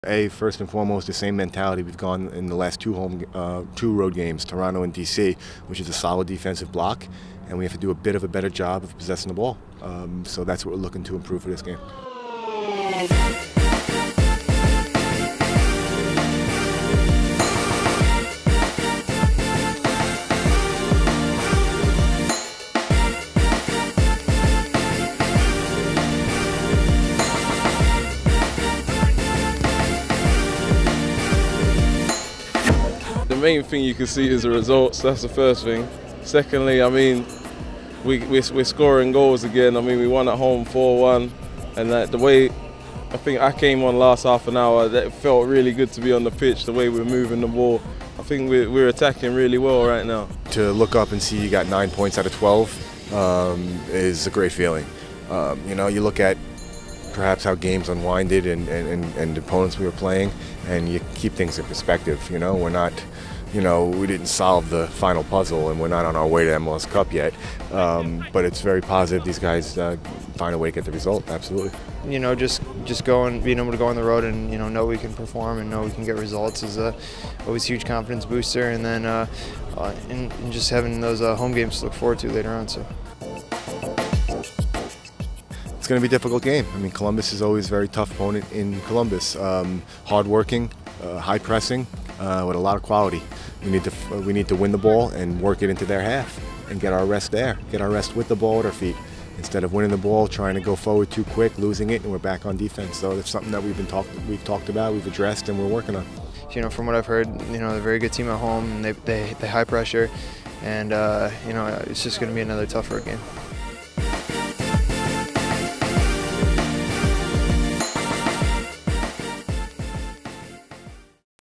Columbus Crew Preview
Mike Petke, Lloyd Sam, and Eric Alexander preview Saturday's match against the Columbus Crew.